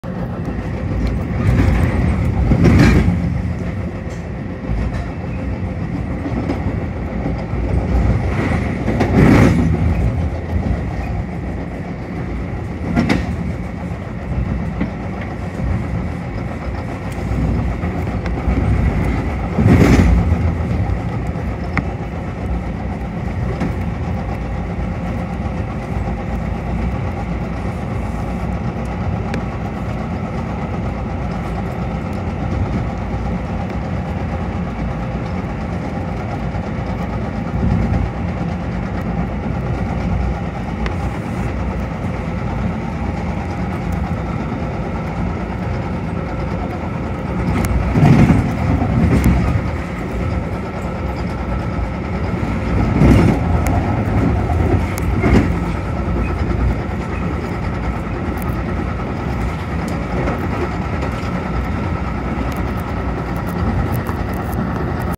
Metra Train Rear View Cab Car Ride Through Barrington Coach Yard On May 11, 2024
I caught a Metra Train Rear View Can Ride through Barrington Coach Yard at 2:33 PM.